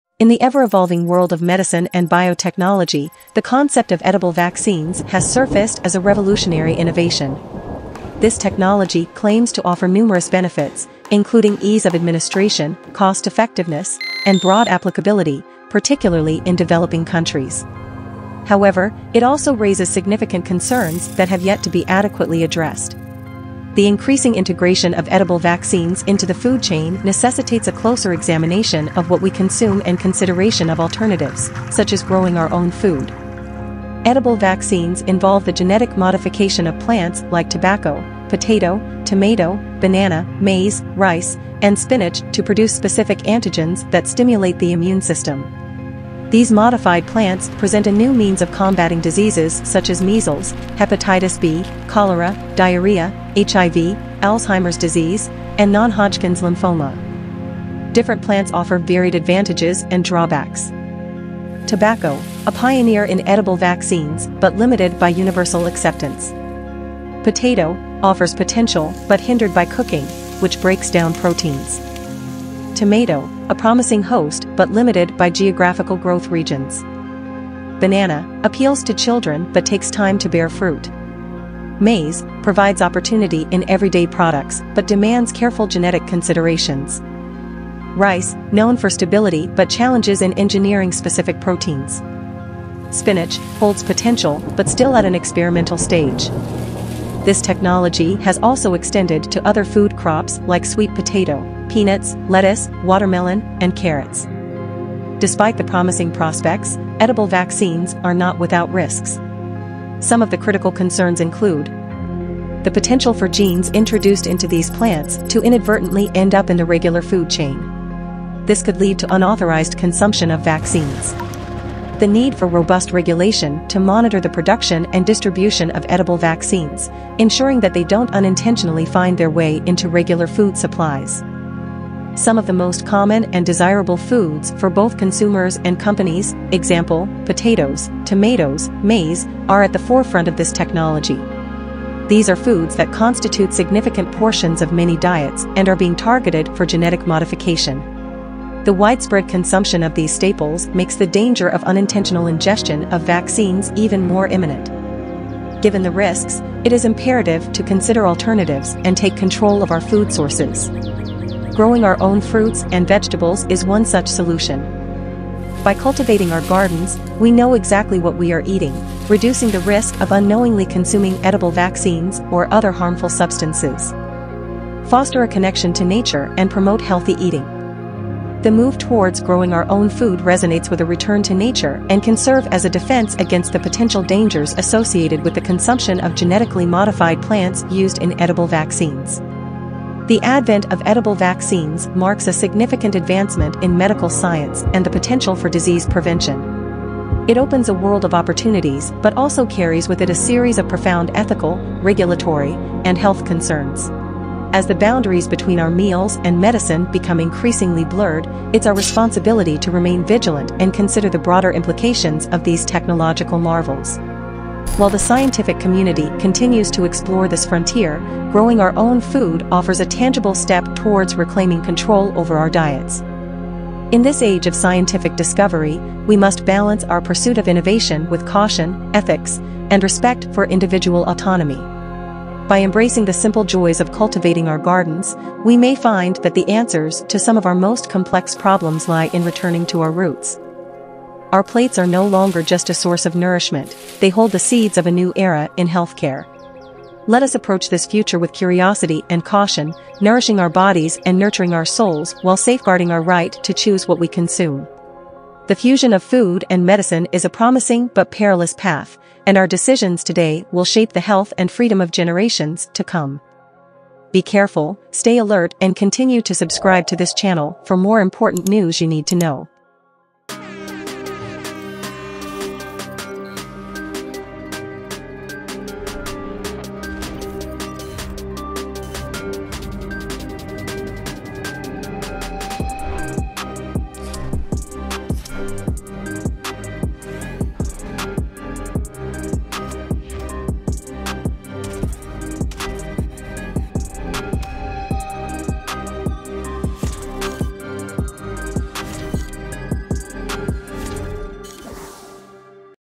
YOU Need To Know About Edible Vaccines - 2 STRONG -
Edible vaccines involve the genetic modification of plants like tobacco, potato, tomato, banana, maize, rice, and spinach to produce specific antigens that stimulate the immune system. These modified plants present a new means of combating diseases such as measles, hepatitis B, cholera, diarrhea, HIV, Alzheimer's disease, and non-Hodgkin's lymphoma.

Fair Media
Granger, Indiana, 46530